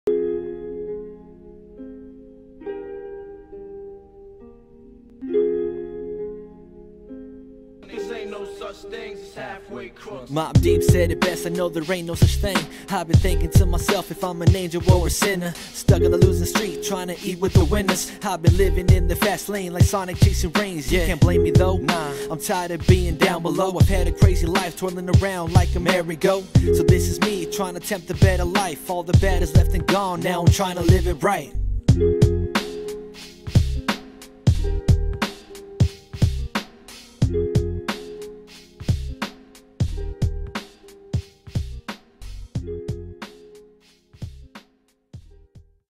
Rhythm J - 8 Bars Producer Wirebeats work mexican rapper dallas texas boombap oldschool beat collab
What's going on YouTube? Here are 8 bars for you to enjoy.
Want a collaboration or a feature? My charge is $20/8 bars (the same amount as recorded in the video.) THANK!